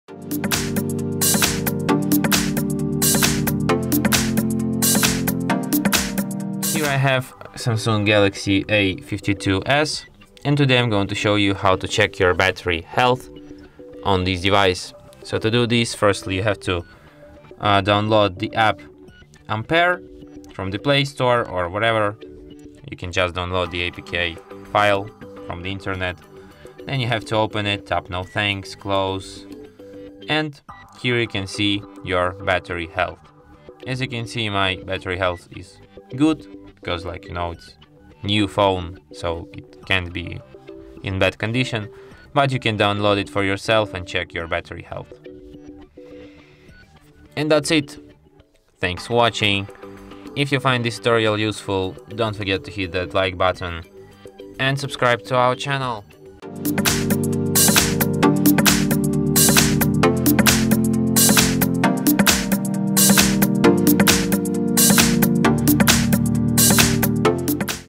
How to Check Battery Health on SAMSUNG Galaxy A52s – Ampere App
Read more about SAMSUNG Galaxy A52s:
In this video, we will show you how to use the Ampere app on your SAMSUNG Galaxy A52s, which will allow you to see detailed information about the battery state of the smartphone. Check the condition of the battery by following the video. The app provides some hidden information about the battery, which some of them can’t be accessed by default. Follow the instructions to learn how to download, install and use the Ampere app on SAMSUNG Galaxy A52s. Repeat all the steps presented in the video guide to start using the Ampere app and to check the state of the battery of your SAMSUNG Galaxy A52s.